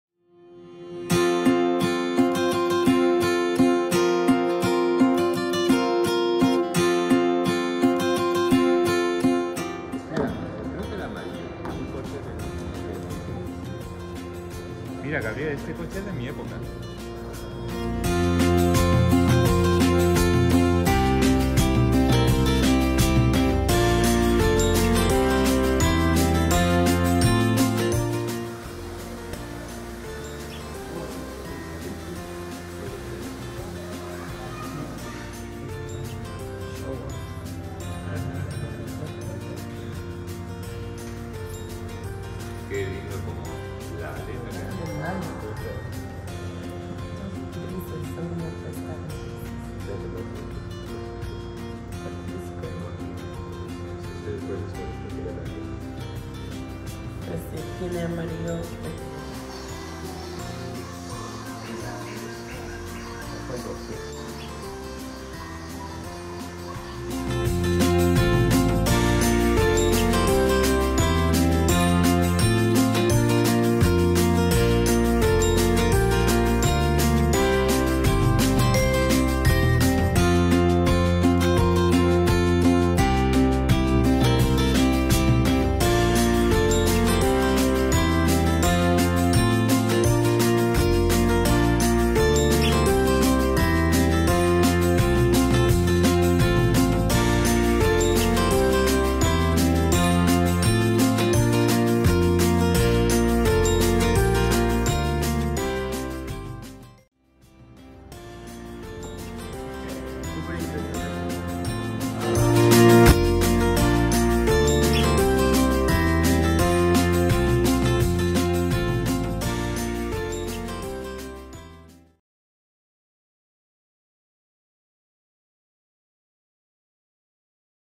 Visit Spain! - Granada, Parque de las Ciencias
This is the first of a series of videos about countries where you can practice your Spanish. We take you through different places and cities that you can visit.